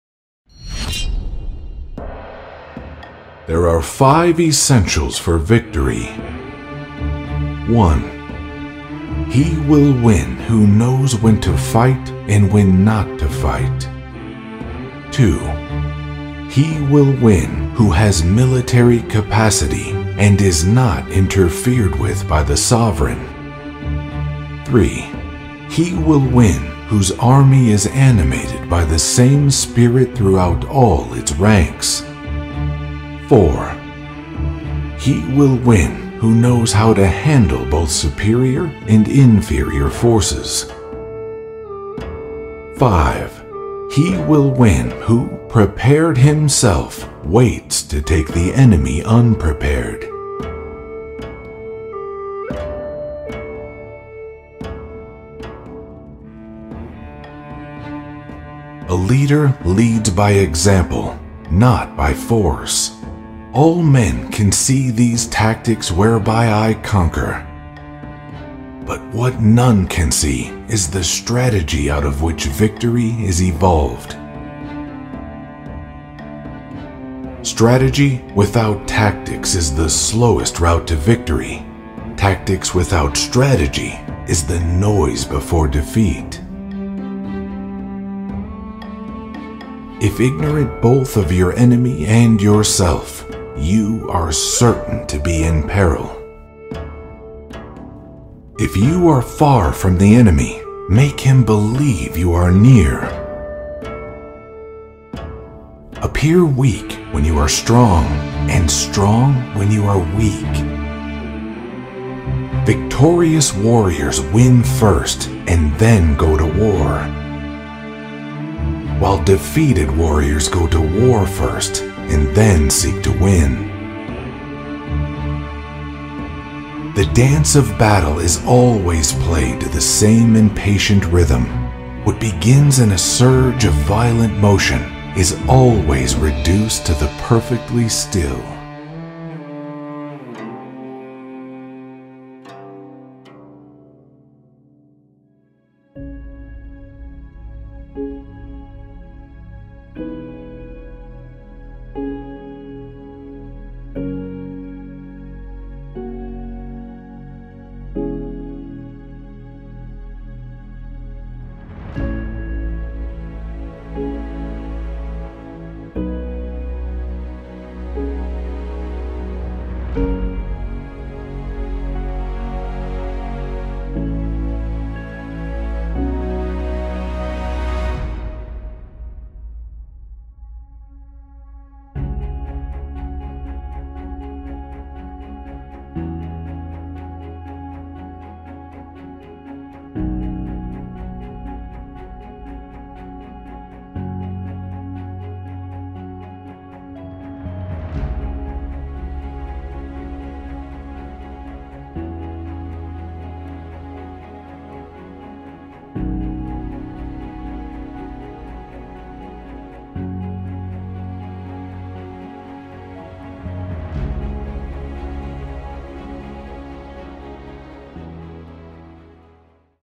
ART OF WAR: Sun Tzu Quotes to Achieve Victory In Your Next Battle
ART OF WAR: Sun Tzu Quotes to Achieve Victory In Your Next Battle. These are some of the Greatest Sun Tzu Quotes EVER on how to win every battle life throws your way! This is the 2nd video to the Sun Tzu series, who was an acclaimed strategist, general, and the author of The Art of War. He established 5 Essential Rules to gaining victory in every kind of battle, these rules are still learned to this day.

“The Art of War is of vital importance. It is a matter of life and death, a road either to safety or to ruin.” - Sun Tzu

▶BECOME A MEMBER of Motiversity’s loyal community!

►Speaker: Alan Blazek

►Quotes

Sun Tzu

The Art of War

Sun Tzu (544 BC - 496 BC) was a Chinese general, strategist, philosopher, and writer. Sun Tzu is credited for creating the 5 Essential Rules to victory and The Art of War, which has affected philosophy and strategy thinking across the world.

►Music
Audiojungle